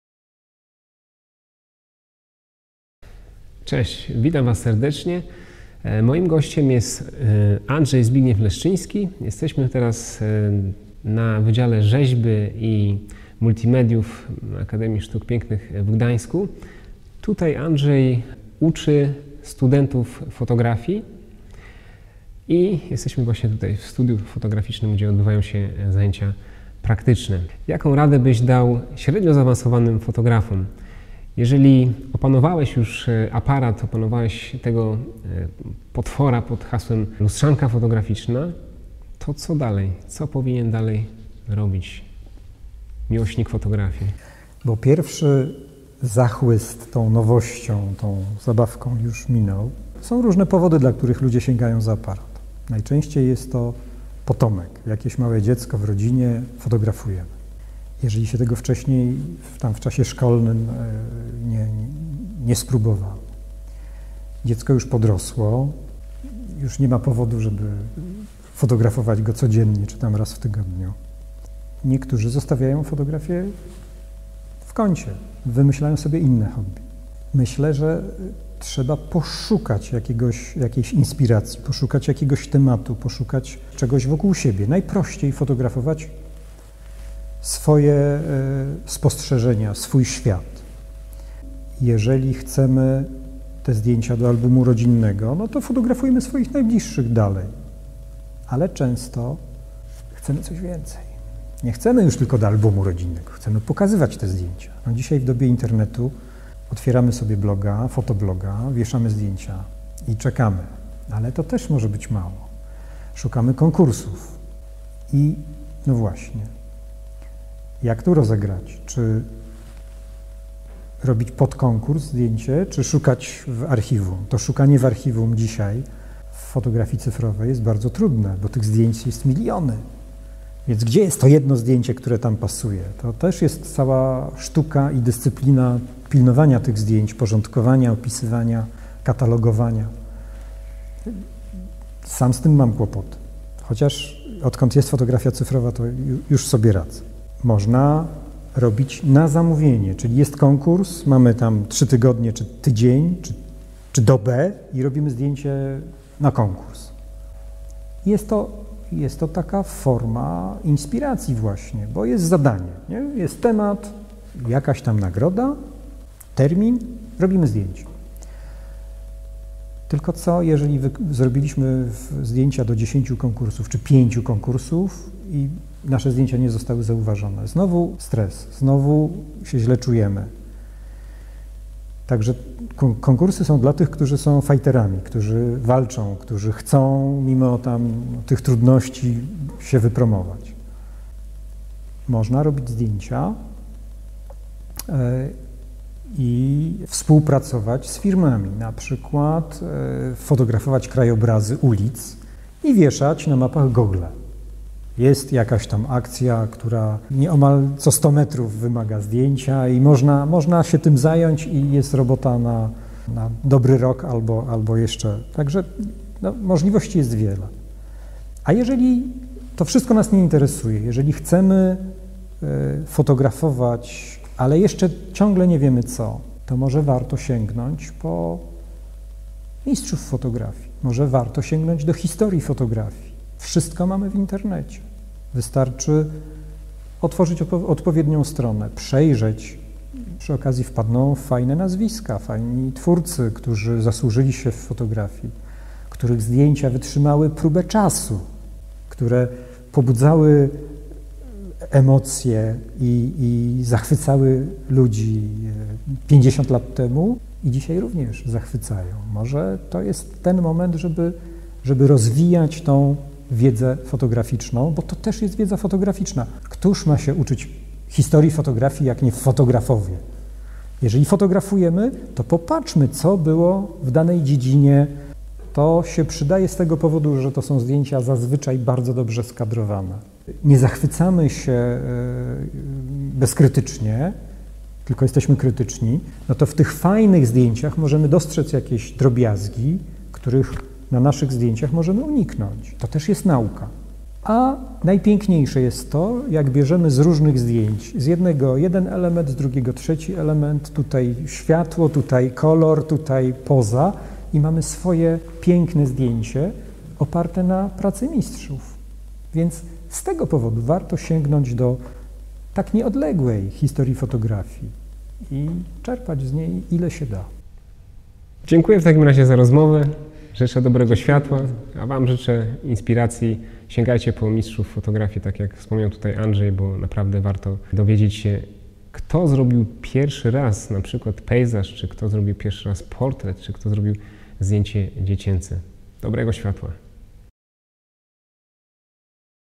Porada fotograficzna dla średnio zaawansowanych fotografów
Gdzie szukać inspiracji? Co dalej robić? Poznałem aparat fotograficzny i stoję w miejscu? Porada fotograficzna dla średnio zaawansowanych fotografów. Wywiad z Andrzejem Leszczyńskim